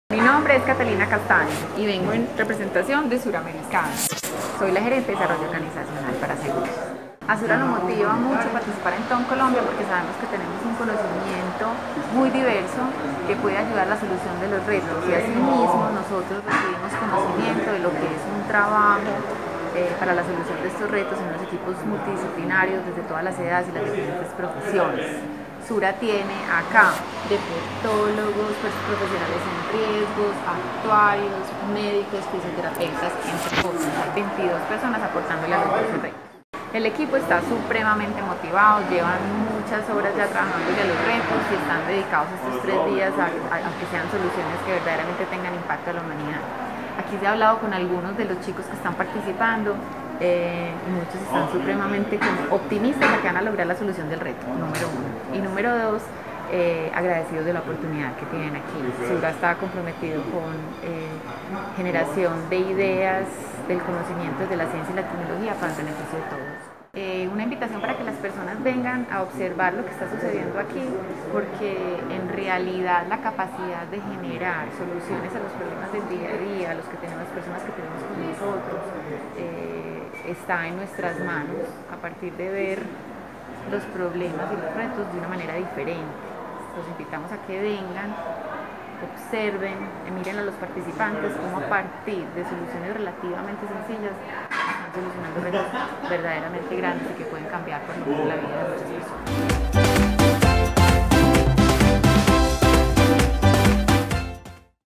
Catalina Castaño - Gerente Desarrollo Institucional SURA
SURA Patrocinador de TOM: Colombia 2017 nos cuenta como ha sido la experiencia de 21 maker de su compañía en este evento de innovación social,  y por que una empresa como SURA se suma a este gran evento.